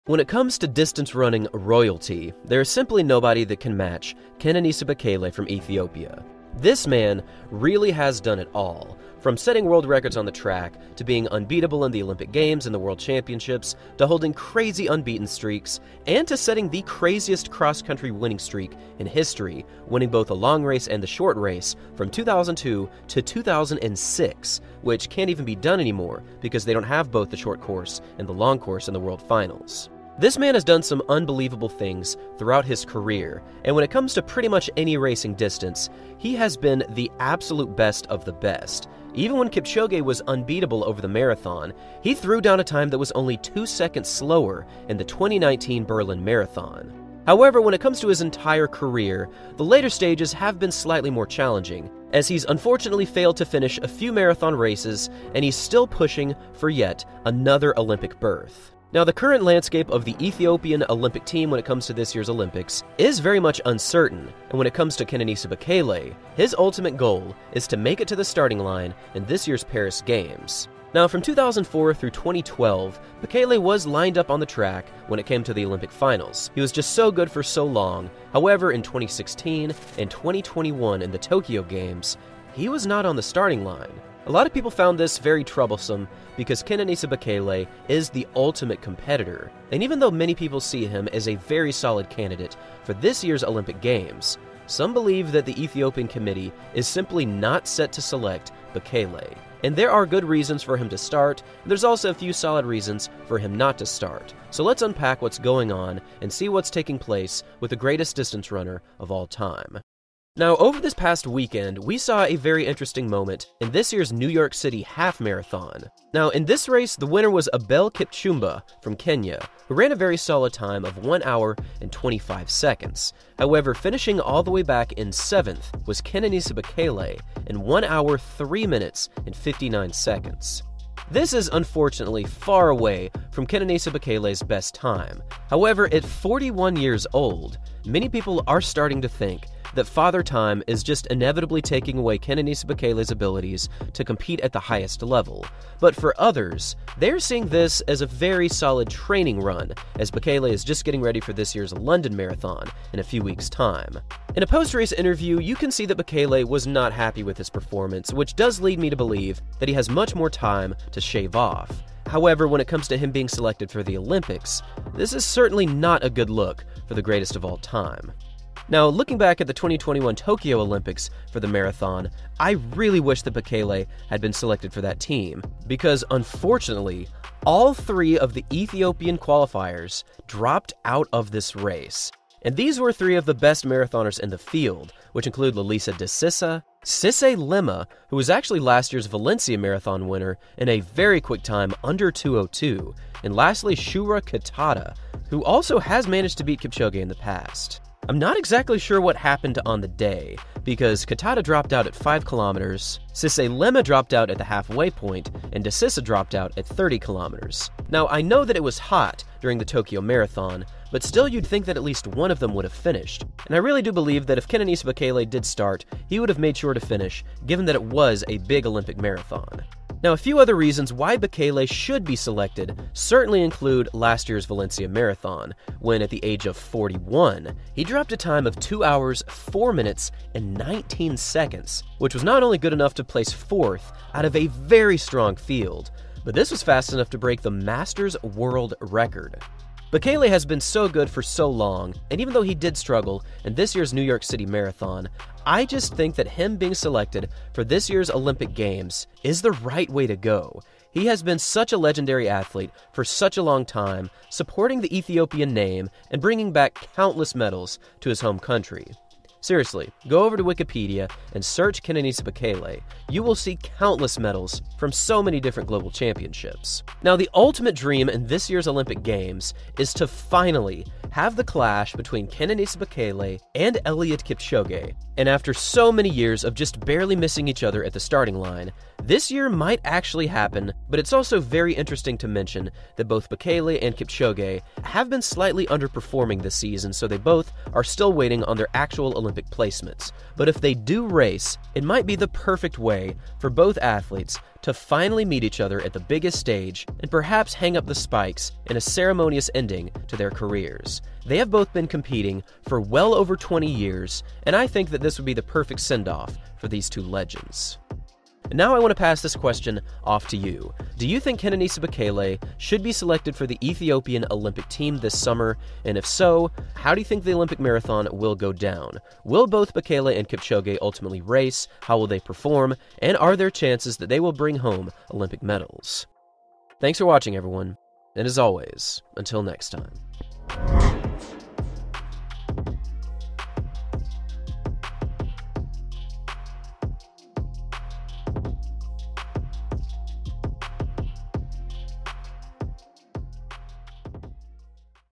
The Kenenisa Bekele Situation
The legend has a date with destiny this summer.  Let's help him get there.